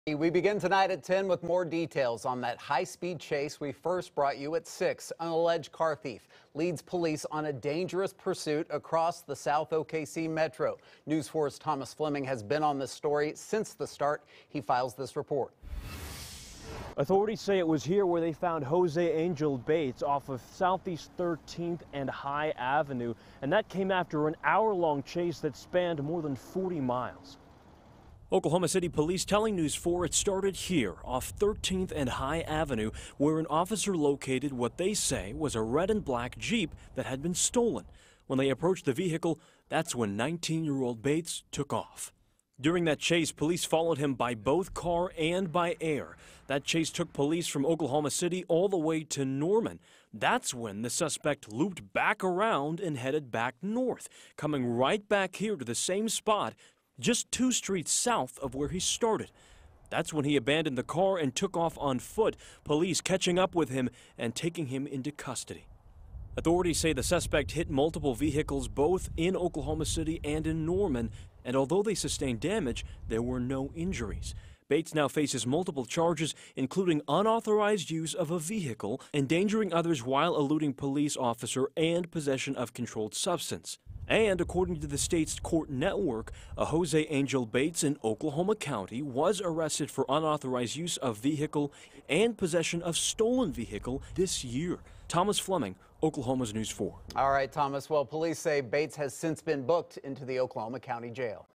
Man arrested after leading police on high-speed chase
A man was arrested after leading police on a high speed chase